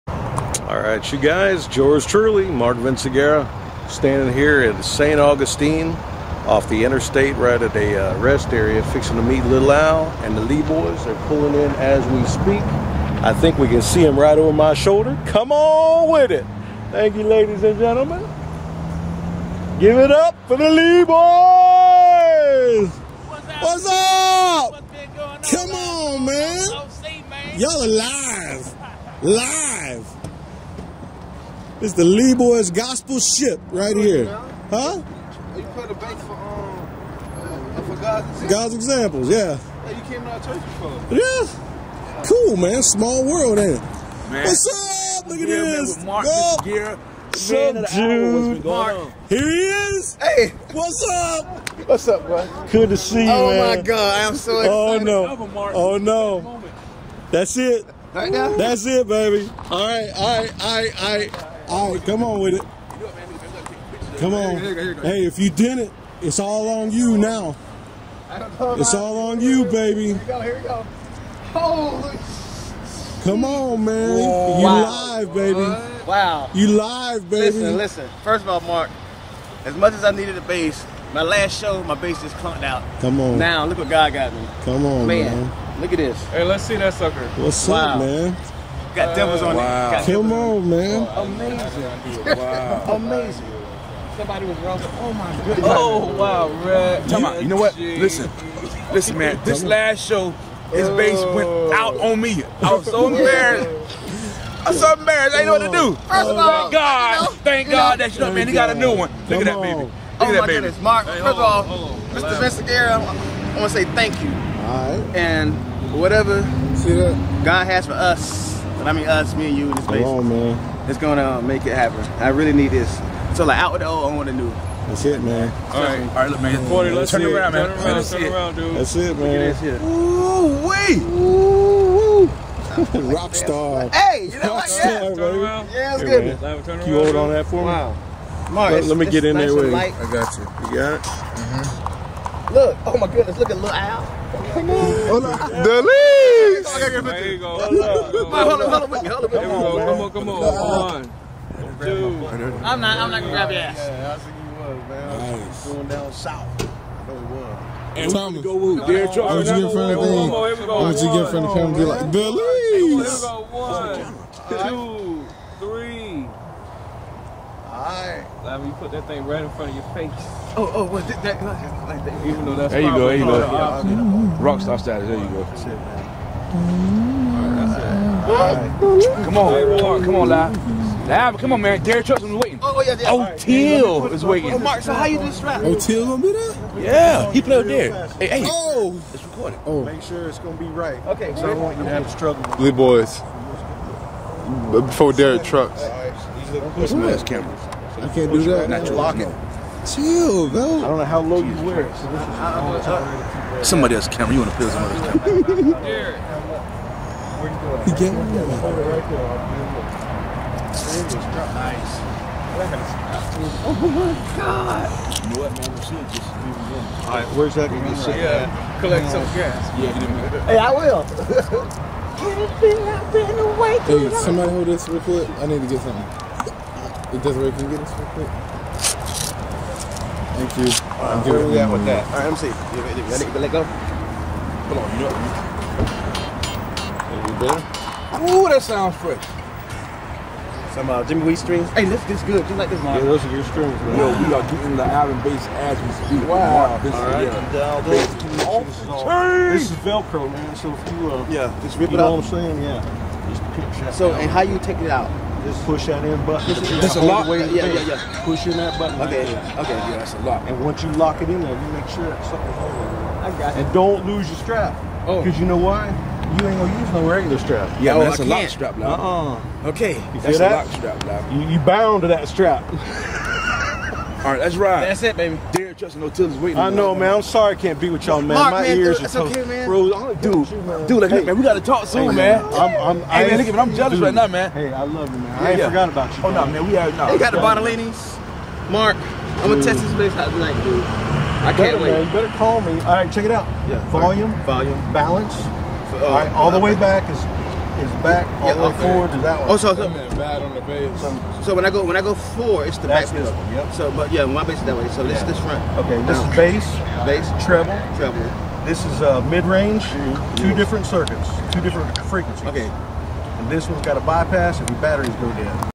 Lil' Al's Bass delivery.mp4
Delivering Lil' Al's new VCS bass. Providence Custom 7